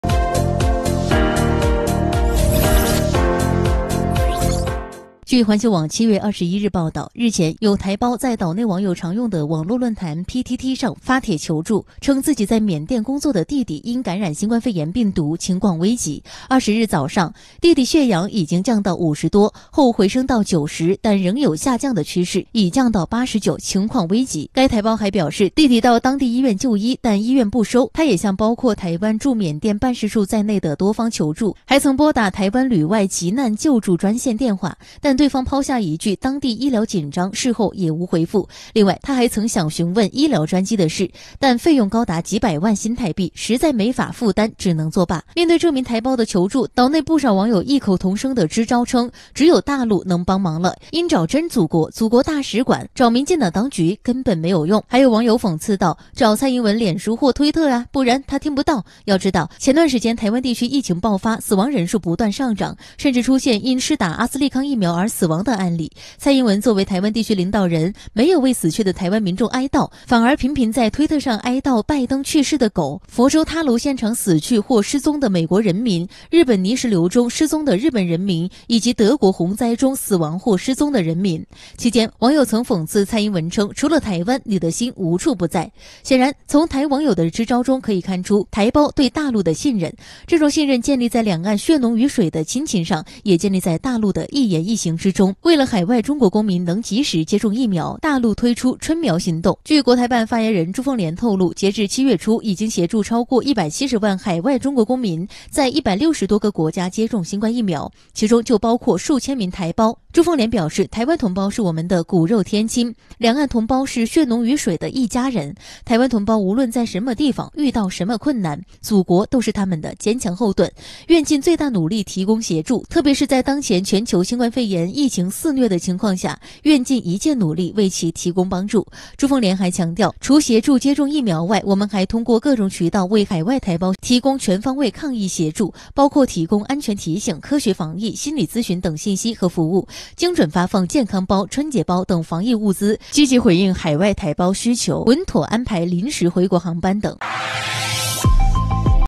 台胞弟弟在缅确诊危急，台当局“见死不救”，网友支招：找真祖国
台胞弟弟在缅确诊危急，台当局“见死不救”，网友支招：找真祖国,本视频由深度解局原创提供,3027次播放,好看视频是由百度团队打造的集内涵和颜值于一身的专业短视频聚合平台